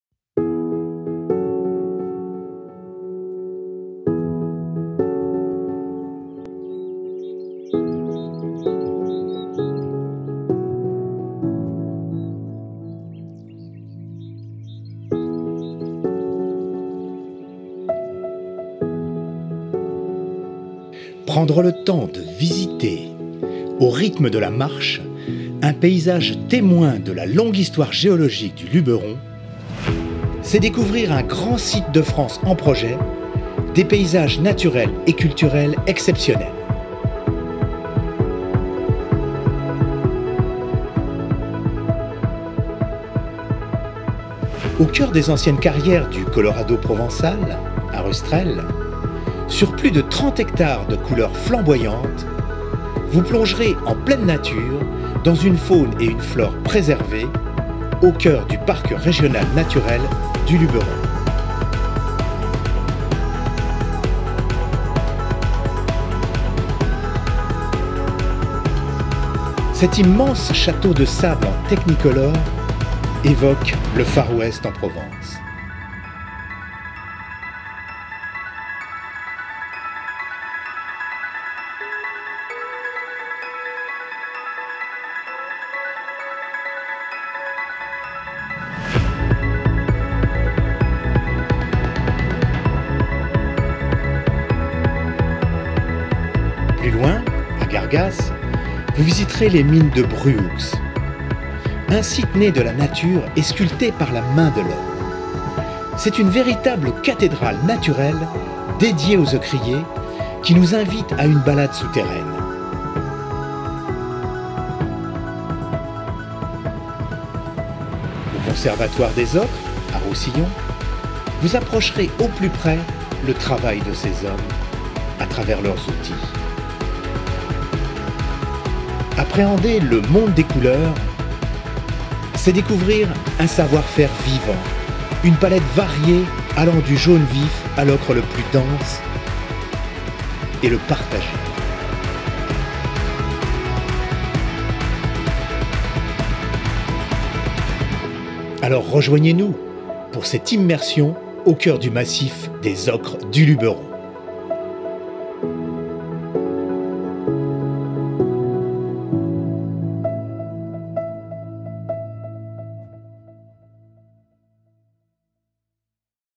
Le Massif des ocres du Luberon
Le massif des Ocres du Luberon, en Pays d'Apt !
Engagé dans une démarche Grand Site de France, le Massif des Ocres a été primé au concours EDEN 2017 (European Destinations of Excellence), distinguant ainsi une destination originale structurée autour du patrimoine ocrier qui a façonné un paysage unique.
Le Massif des Ocres est sans doute l'un des éléments paysagers les plus remarquables de la région du Luberon. L'exploitation humaine depuis une centaine d'années (carrières et mines), conjuguées à l'érosion naturelle, a façonné des sites spectaculaires qui attire la curiosité d'un large public.